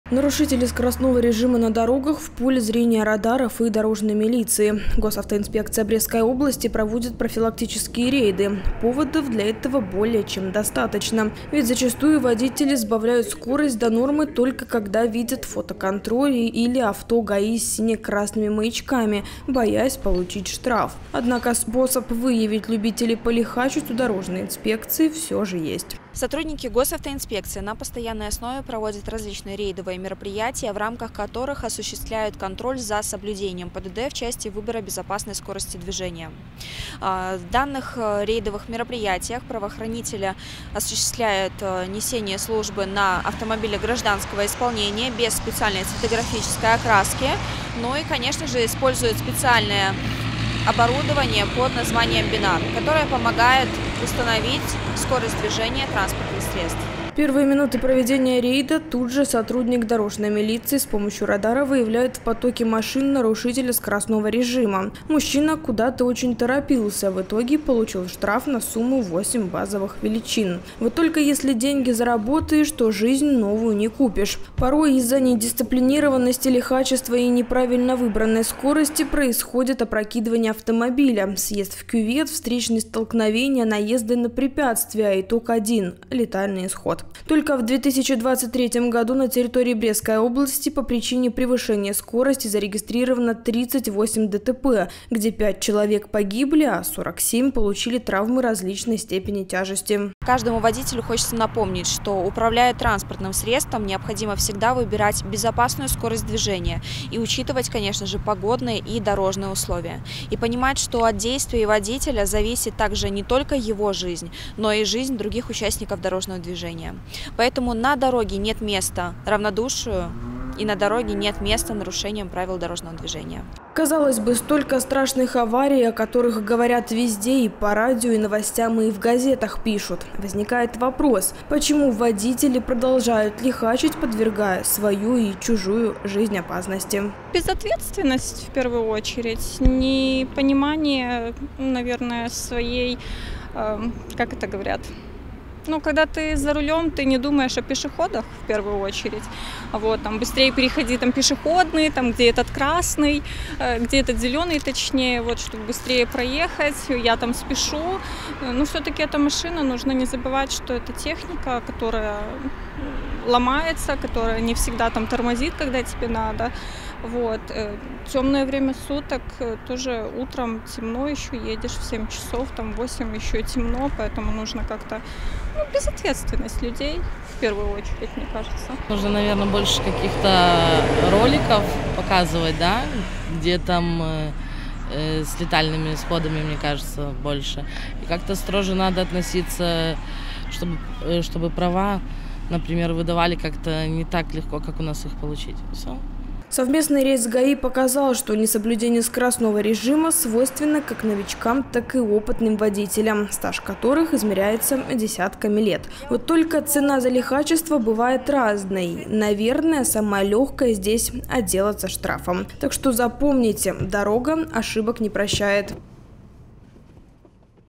2024-02-06 Акция ГАИ: скоростной режим
Безопасность на дорогах зависит от многих факторов, не последнее место в списке которых занимает вежливость и добропорядочность самих водителей и пешеходов. Но порой представители и тех, и других забывают, что сами и отвечают за собственный комфорт и нарушают правила дорожного движения. Одним из безусловных лидеров списка правонарушений на дорогах Брестчины является превышение скорости. С целью предупреждения подобных лихачеств уже не один год в стране проходят специальные рейды сотрудников ГАИ. В очередной рейд отправилась совместно с правоохранителями отправилась и наша съёмочная группа
*Наши контакты:*
✅Телефон горячей линии:
54-63-67